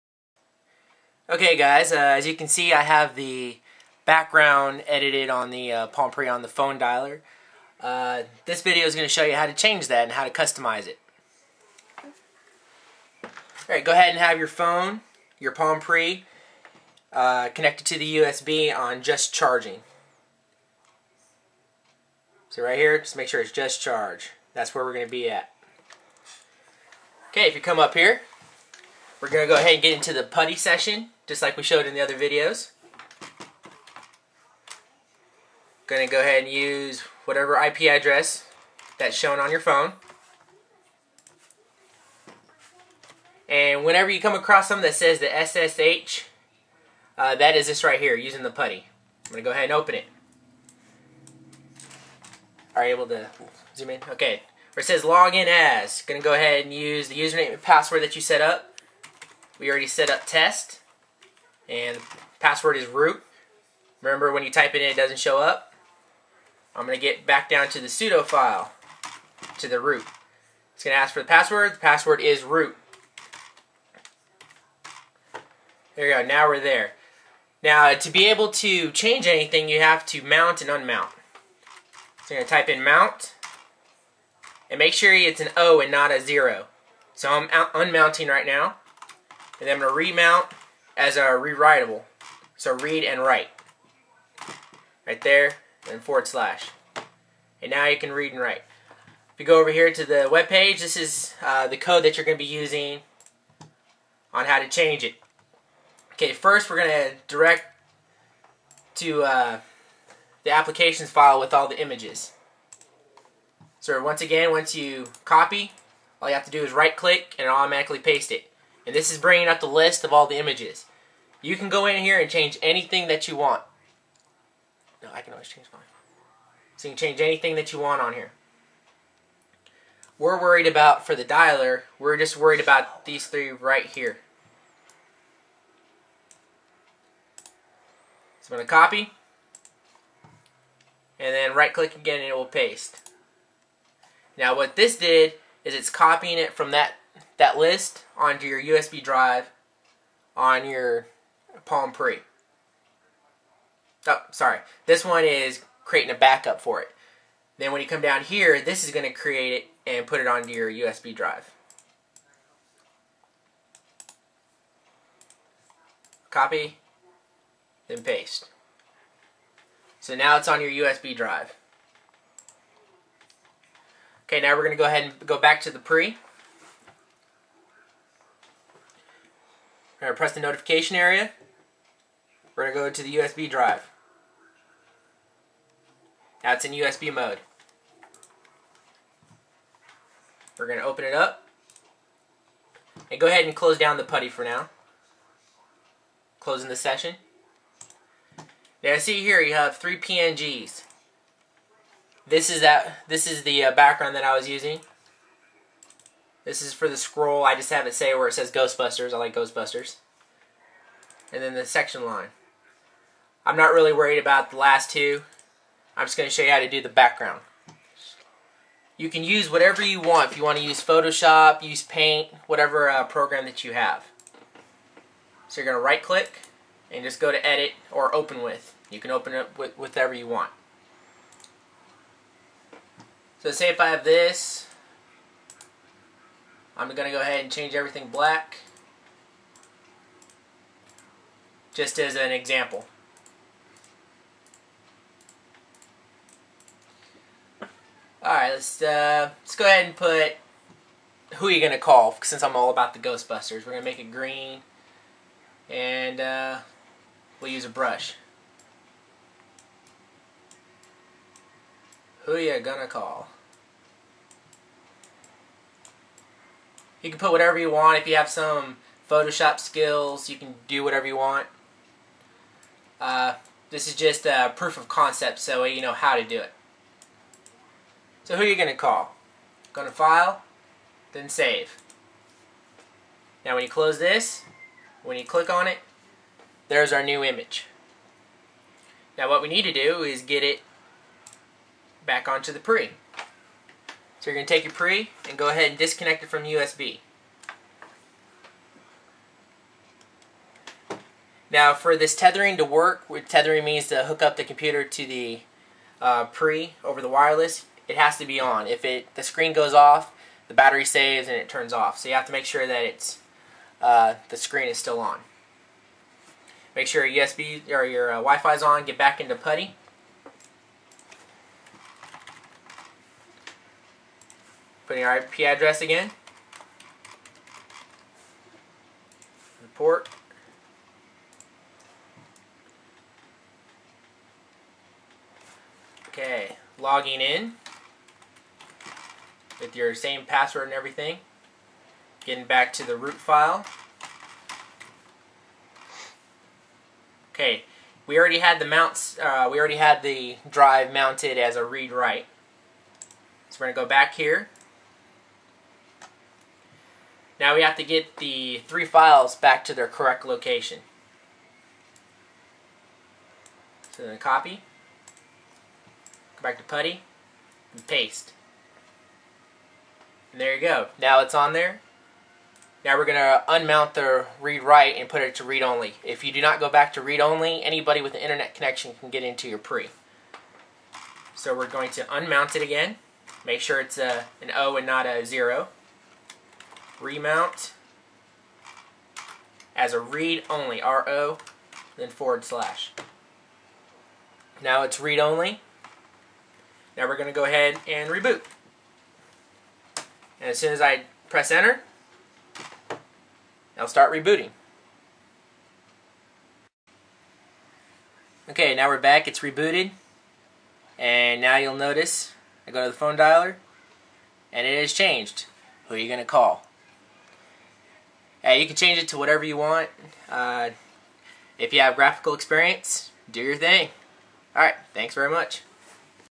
How to change background and themes for phone dailer on Palm Pre
Make sure your pre is rooted

Just proof of concept type of thing.